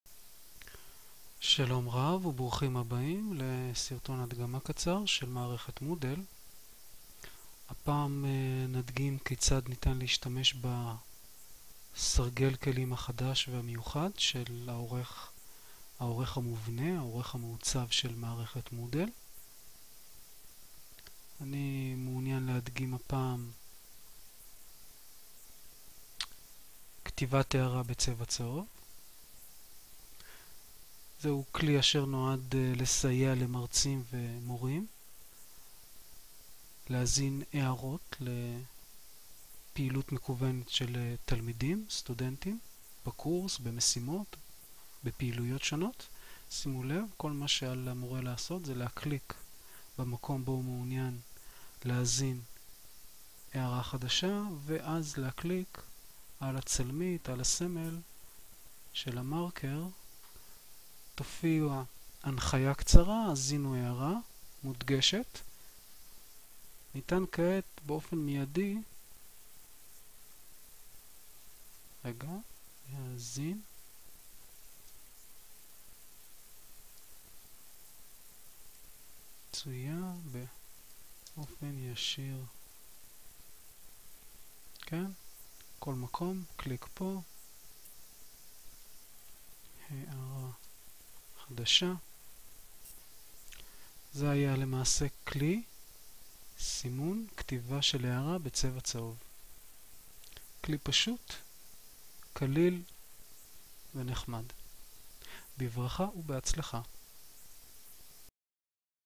הוספת הערה מודגשת בעורך המעוצב
הוספה של הערה מודגשת בעורך המעוצב של מערכת מוודל